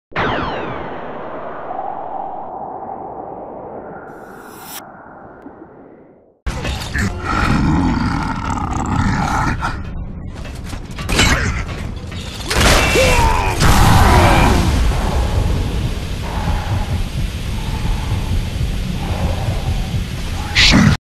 Shenanigans 15 - Try And Smoke Tag Me Will You? | SharixStudios
This was recorded during the One Shot One Kill Weekend( I do miss that Playlist). Taught him to watch out who he tries to smoke tag down d: . Enjoy